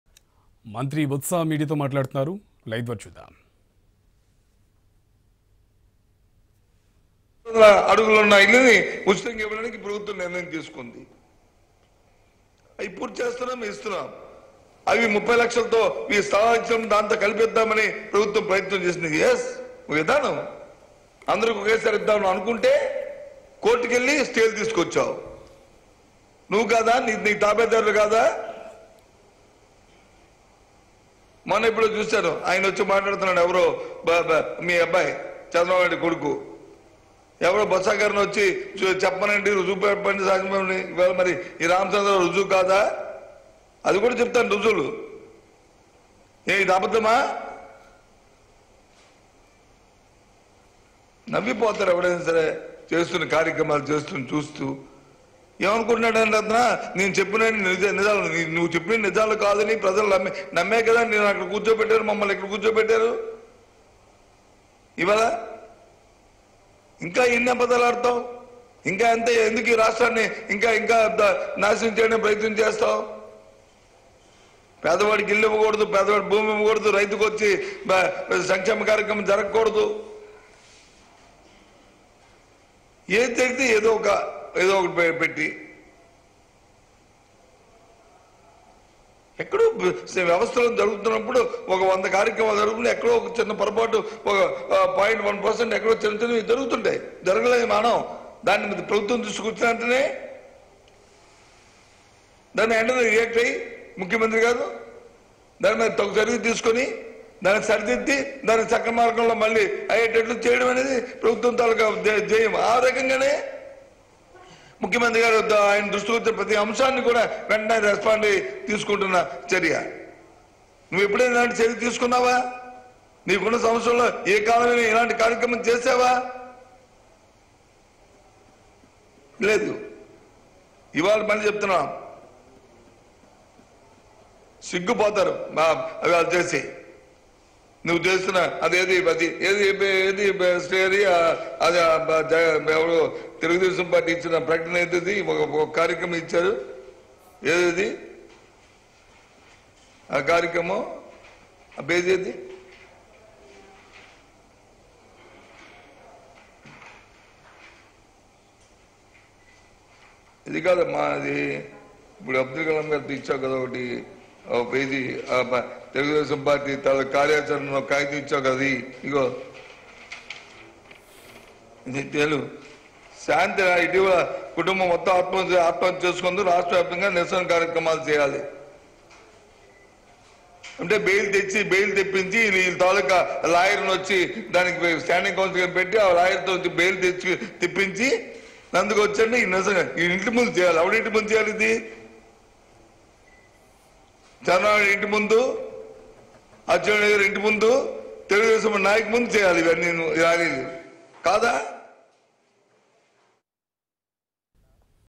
Minister Botsa Satyanarayana Slams Chandrababu and Nara Lokesh | Over House Patta Distribution
Minister Botsa Satyanarayana Slams Chandrababu and Nara Lokesh | Over House Patta Distribution | iNews